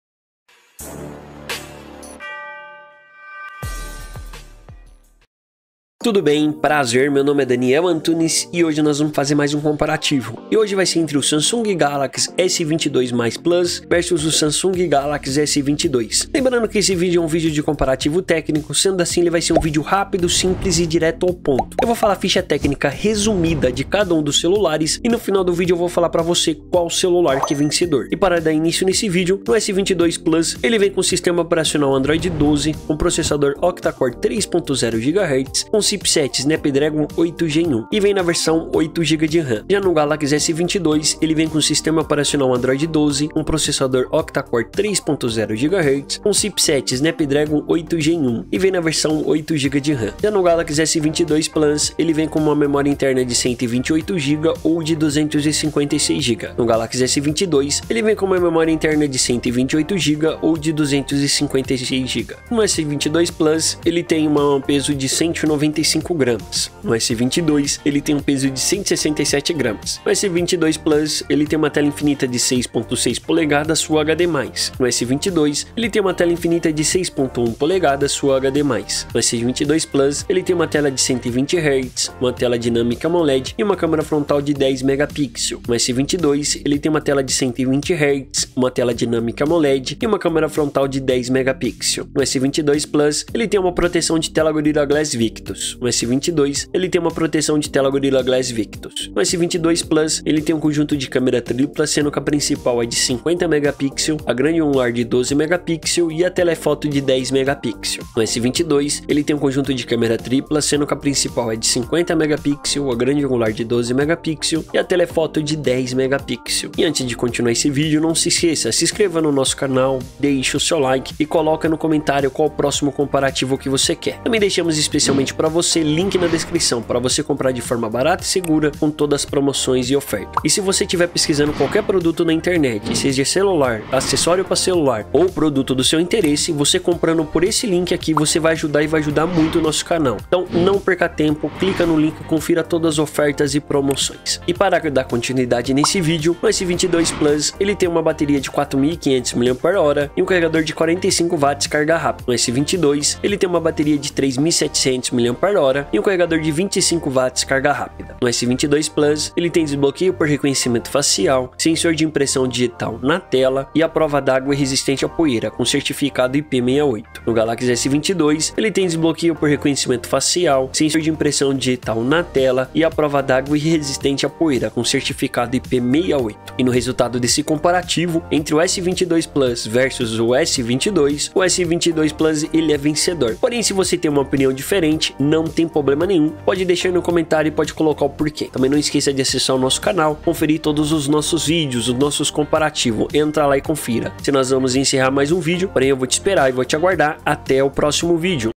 Galaxy S22+ Plus 5G vs Galaxy S22 5G [COMPARATIVO]
Qual é o Melhor Galaxy S22+ Plus 5G vs Galaxy S22 5G?

Neste vídeo vamos fazer um comparativo técnico e vamos falar qual o celular vencedor.

Galaxy S22+ Plus

Galaxy S22

🔻🔻Produtos mais vendidos na AMAZON🔻🔻

Redmi 9

Redmi Note 11s

IPhone 11

IPhone 12

Galaxy M53